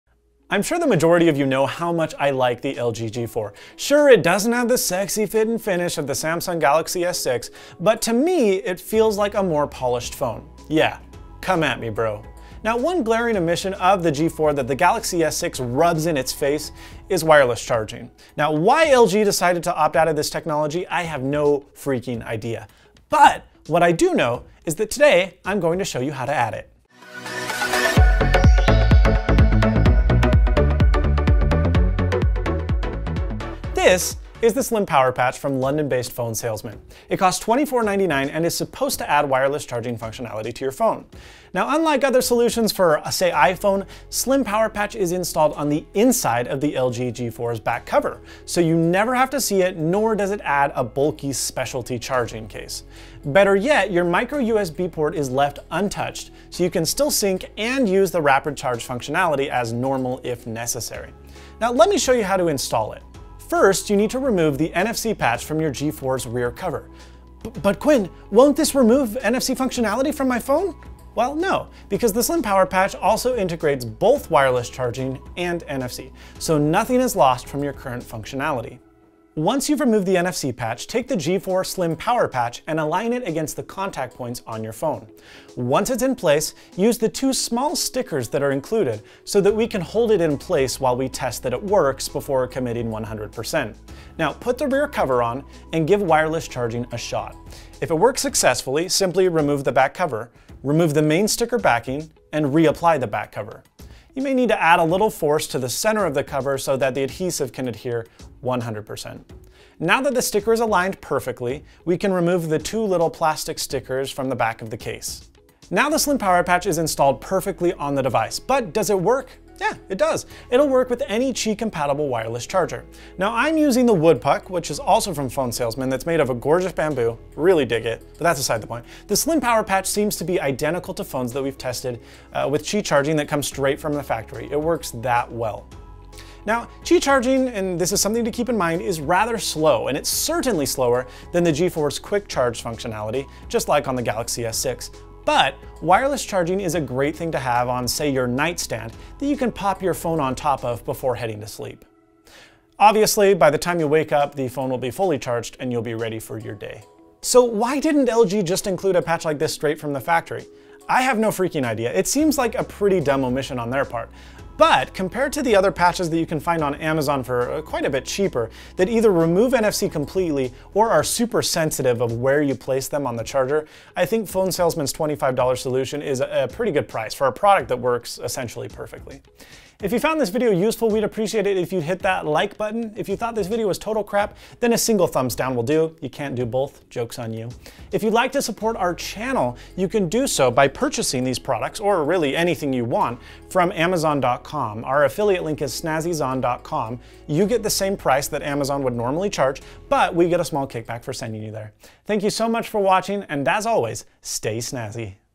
Add Wireless Charging to your LG G4 with SlimPWRPatch
Link to SlimPWRPatch
One of the biggest omissions in the LG G4 was the lack of wireless charging. A few solutions have popped up on the Net, but none of them are very elegant or well-made. Fonesalesman thinks they have a solution worthy of the $24.99 price tag, but is theirs any better than the cheap alternatives on Amazon? Watch to find out.